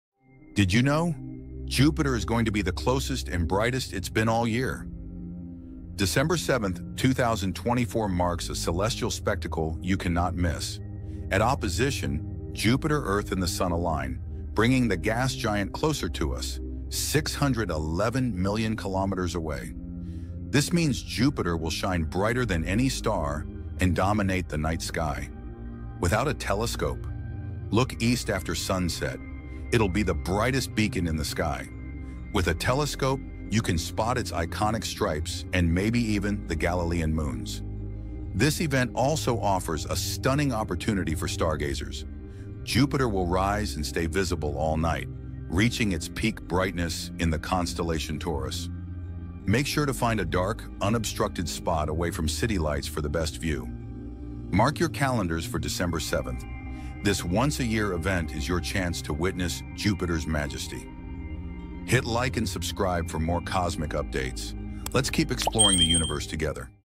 Jupiter making closest approach to Earth | December alert
Get ready, stargazers! On December 7th, 2024, Jupiter comes closer to Earth than it will for the rest of the year! At opposition, this gas giant will shine brighter than any star in the night sky, making it the perfect time to spot its vibrant details—even with just binoculars! 🌠

Whether you’re an avid astronomer or a casual stargazer, this celestial event is a must-see. Learn how to spot Jupiter, what to expect with or without a telescope, and why this alignment is so special. 🚀✨